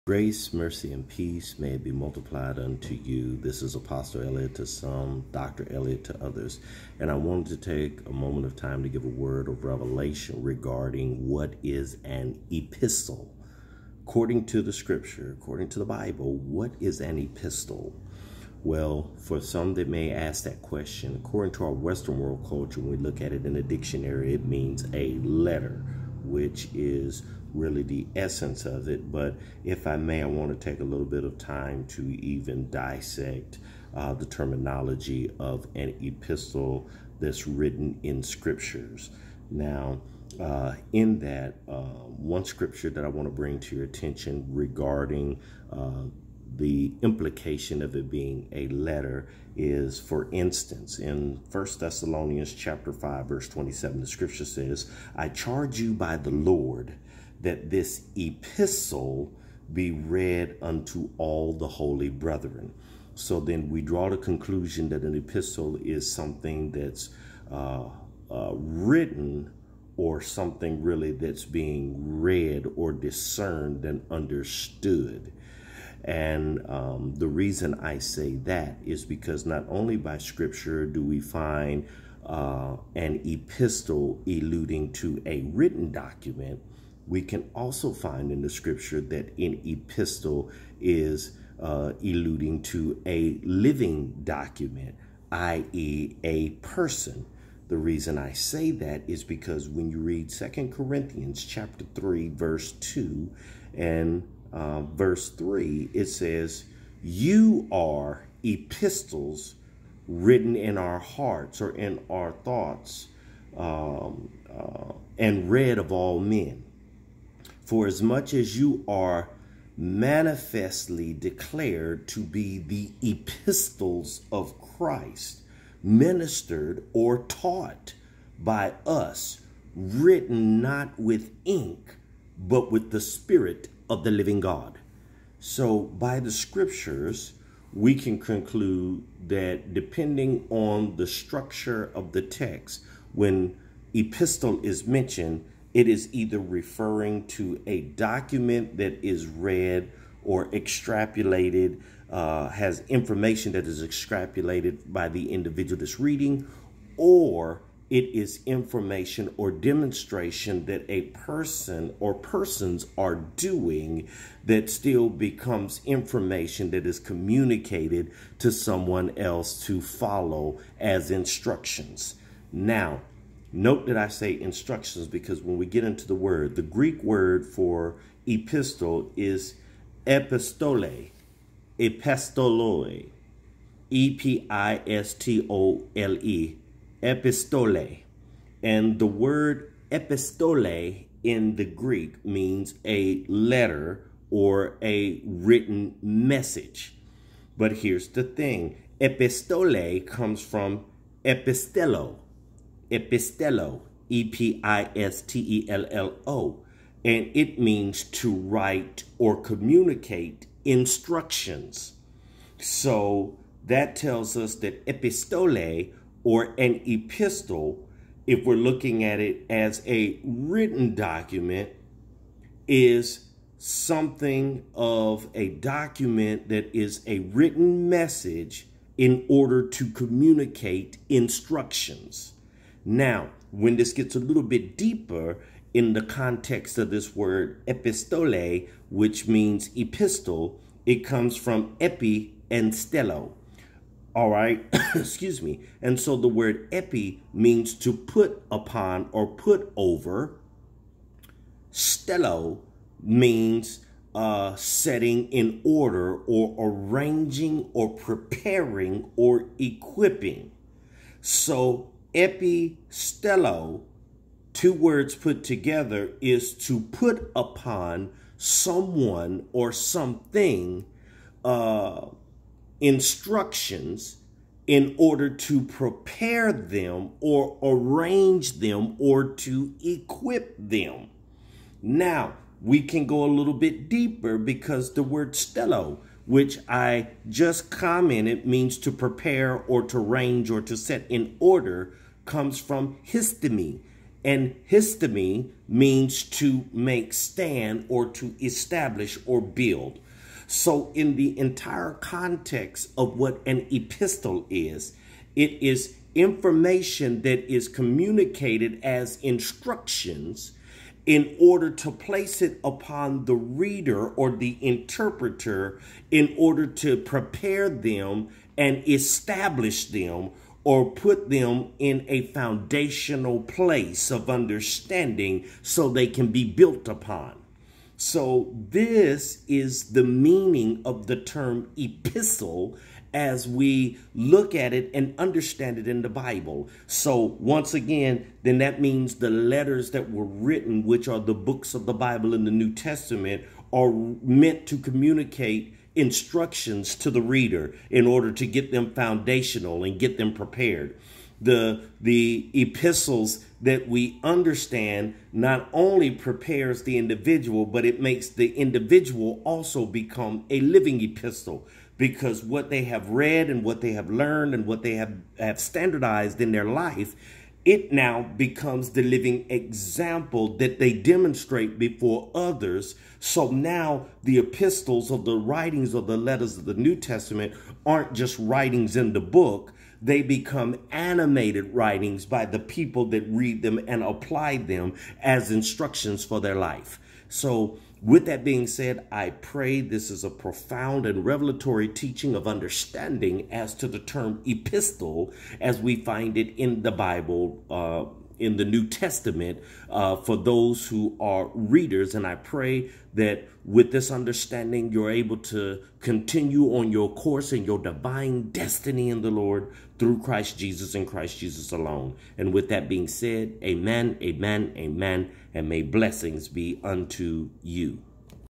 Epistle: A Biblical Perspective
Teaching on the meaning of the word "epistle" as it appears in the Bible in the Greek language.